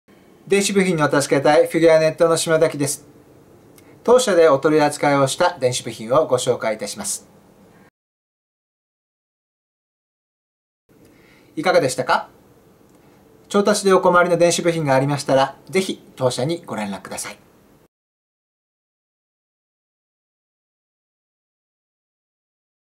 電子部品の調達事例：部品型番LT9010E 製造元 N/A【提供フィギュアネット】
電子部品を世界から調達しているフィギュアネットの調達事例
部品の型番はLT9010E
電子部品の調達に困ったら　弊社までご覧楽下さい。